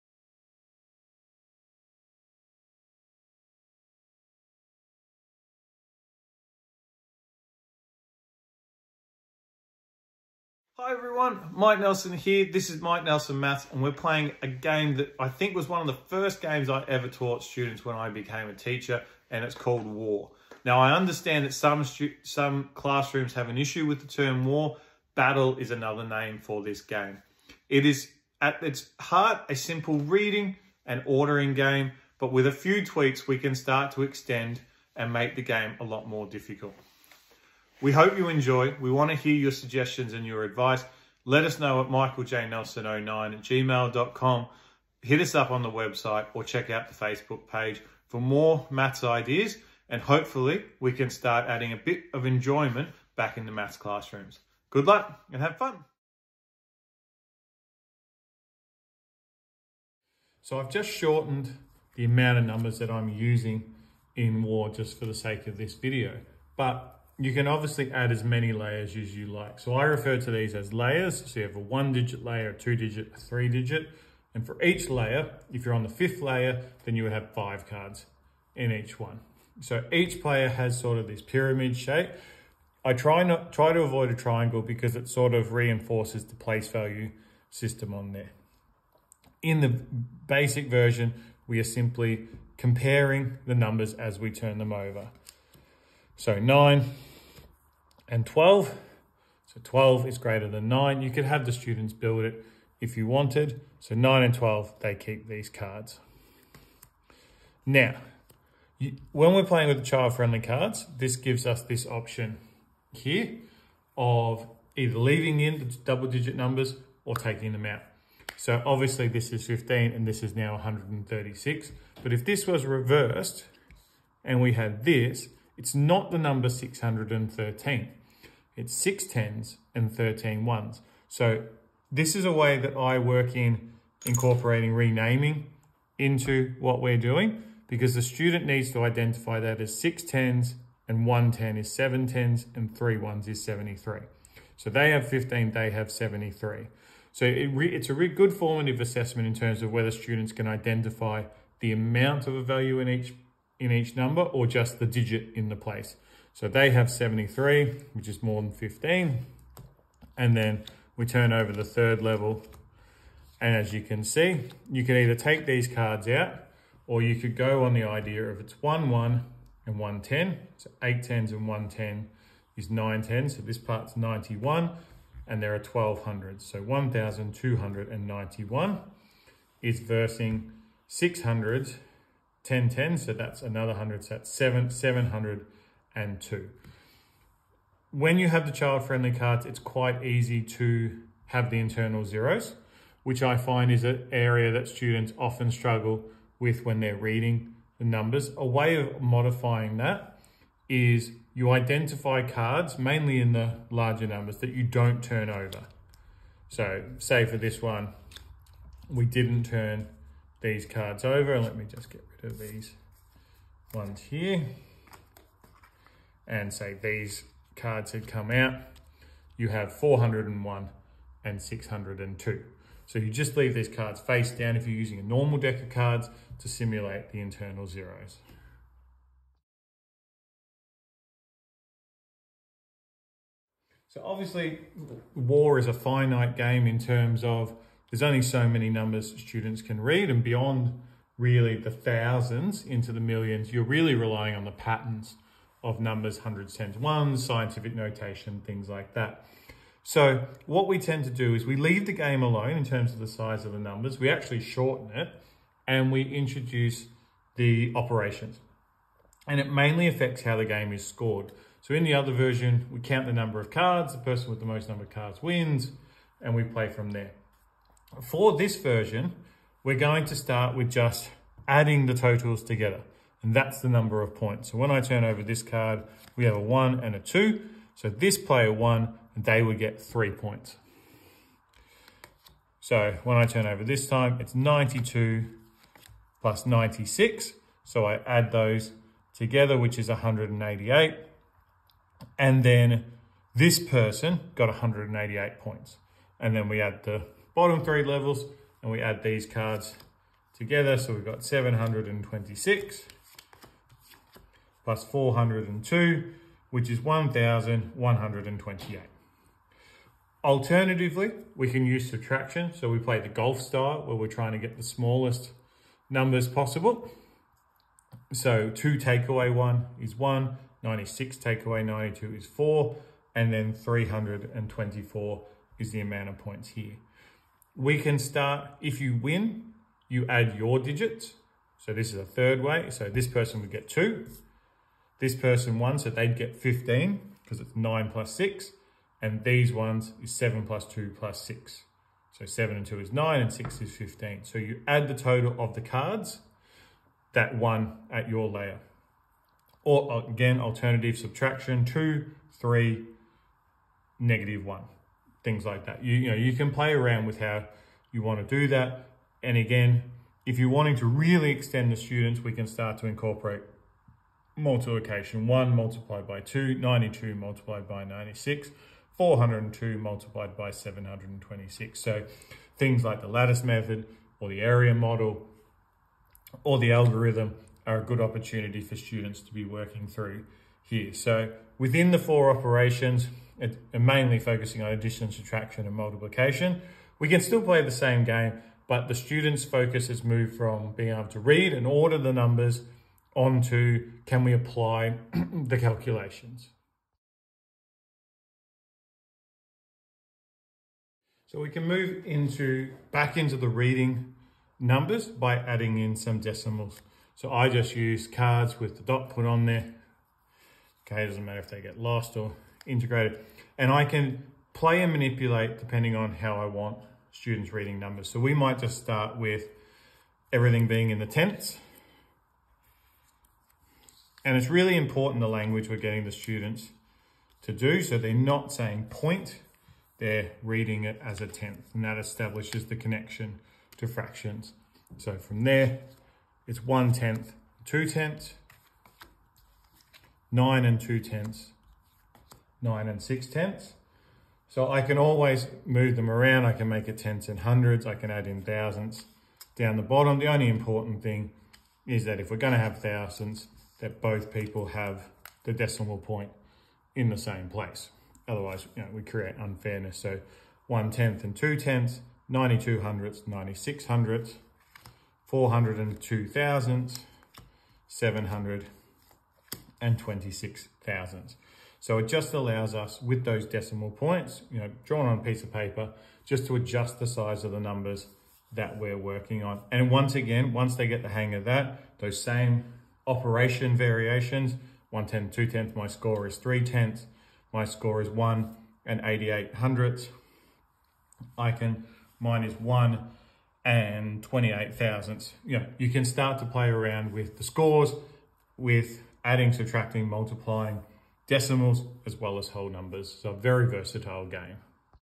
presents War (Battle) (reading numbers, addition, subtraction, multiplication)
War (Battle) is a very easy game to play but an incredible flexible game, with students able to
practice reading, ordering, addition, subtraction, multiplication and decimals.